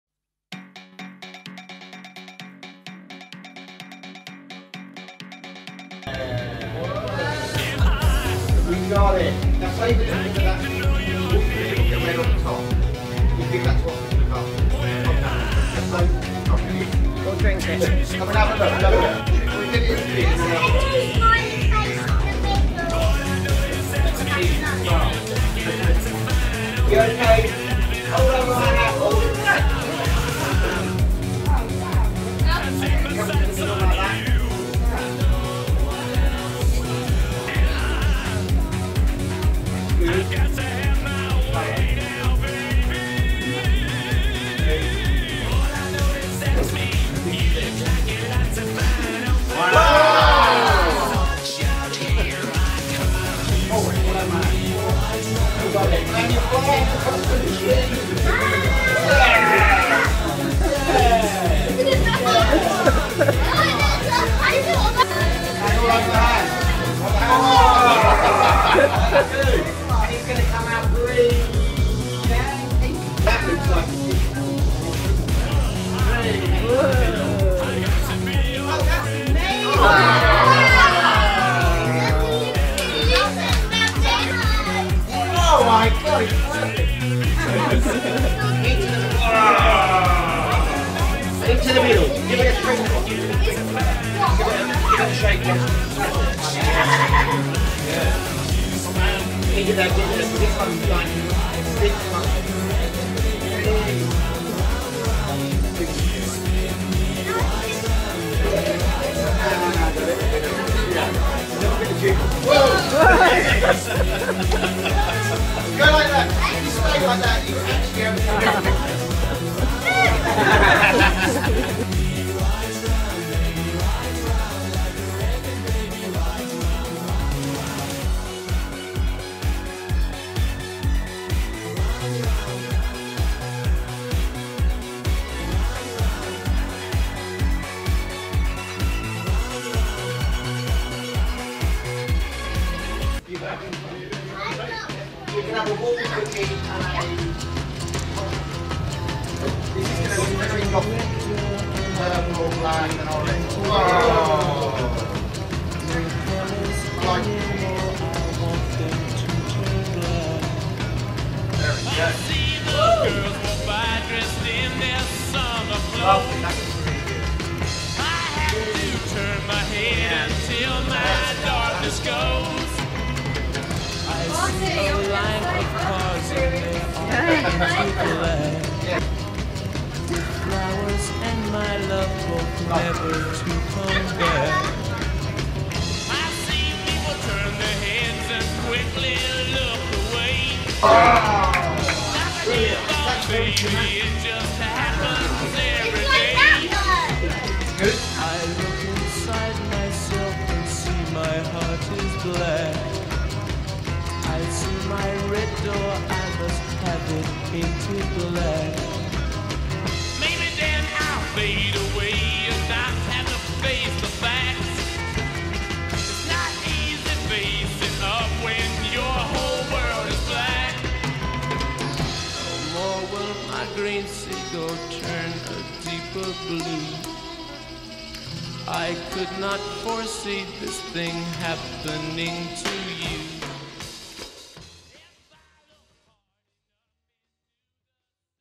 Lloyd's splatter party
Colour Makes People Happy… and it certainly makes them messy too. We visited a quirky paint shop in South London this weekend for a 5-year-old’s birthday party (Eddie’s cousin Lloydy, no less). A strange venue for a children’s party, you might think, but walk through the wonderful world of colour and clogs to the very back of the shop and you’ll discover a crazy spinning painting machine.